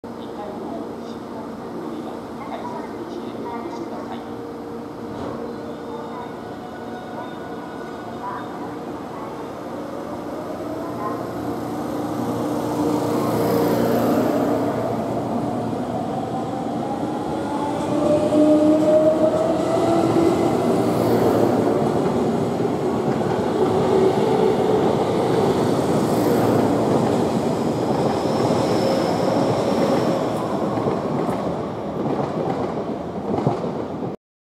813系(9両) 普通 門司港行(2392M) 久留米駅発車
普通 2392M(土曜・休日運転)　荒尾6:25→門司港10:06

※2016年土曜・休日ダイヤ